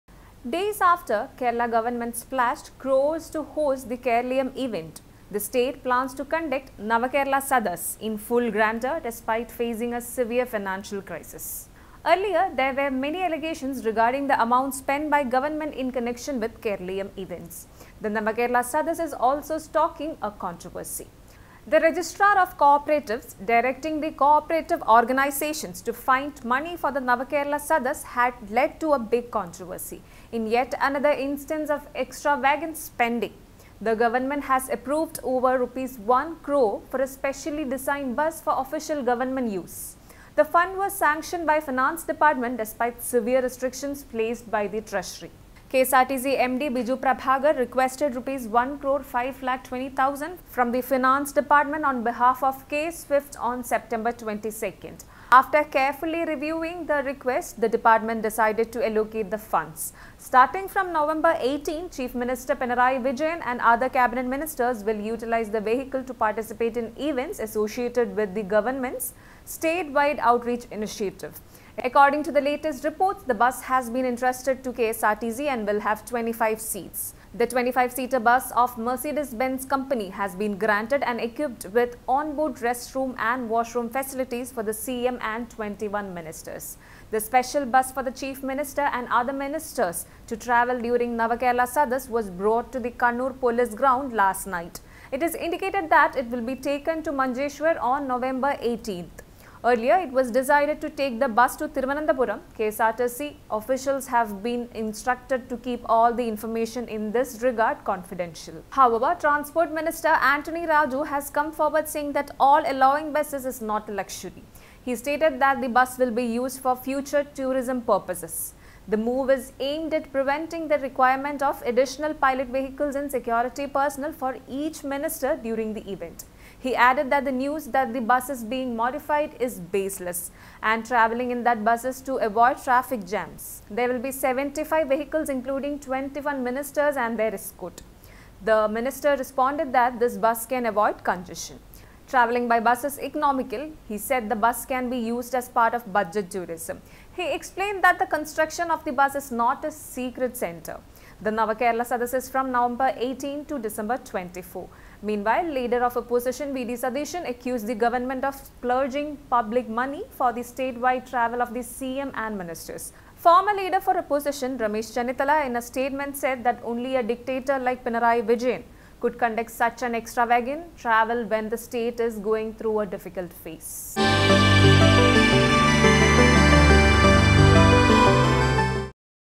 Yet another instance of extravagant spending
The government of Kerala has approved over Rs 1 crore for a specially designed bus for official government use. The fund was sanctioned by the Finance Department despite severe restrictions placed by the treasury. KSRTC MD Biju Prabhakar requested Rs 1,05,20,000 from the finance department on behalf of K-SWIFT on September 22.